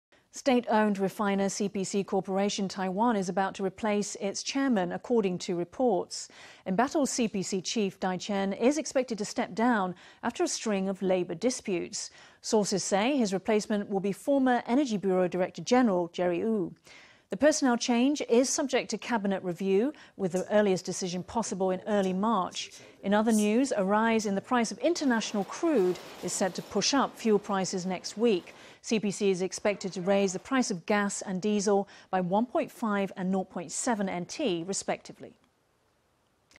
Embattled CPC chairman to step down: report
State-owned refiner CPC Corporation, Taiwan is about to replace its chairman, according to reports. Embattled CPC chief Tai Chein is expected to step down after a string of labor disputes. Sources say his replacement will be former Energy Bureau Director-General Jerry Ou.The personnel change is subject to Cabinet review, with the earliest decision possible in early March. In other news, a rise in the price of international crude is set to push up fuel prices next week. CPC is expected to raise the price of gas and diesel by NT$1.5 and NT$0.7, respectively.